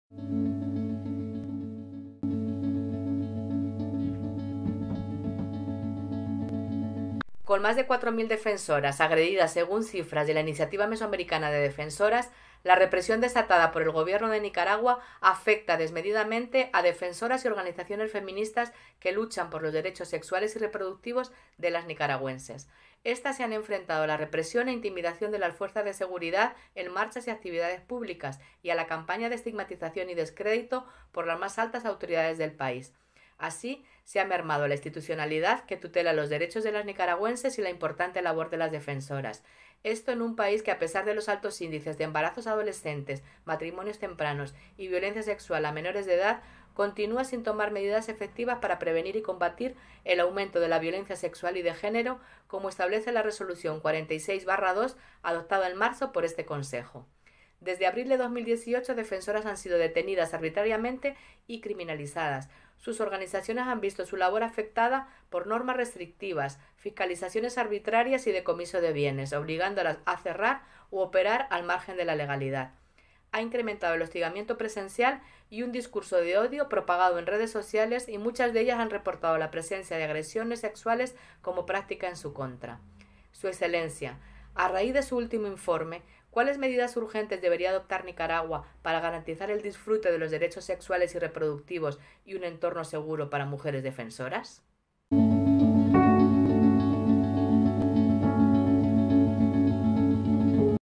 Declaración conjunta de ISHR e IM-Defensoras sobre mujeres defensoras en Nicaragua, 25 de junio 2021
Maria Eugenia Gómez López de la Iniciativa Mesoamericana de Mujeres Defensoras (IM-Defensoras), dió una declaración conjunta por el Servicio Internacional para los Derechos Humanos (ISHR) sobre la situación de las mujeres defensoras de los derechos sexuales y reproductivos en Nicaragua, en el 47o período de sesiones del Consejo de Derechos Humanos de la ONU. La declaración fue pronunciada durante el diálogo interactivo con el Grupo de Trabajo sobre discriminación contra las mujeres y las niñas, y fue respaldada por el Instituto Raza e Igualdad (Race & Equality), y la Red International de Derechos Humanos (RIDH).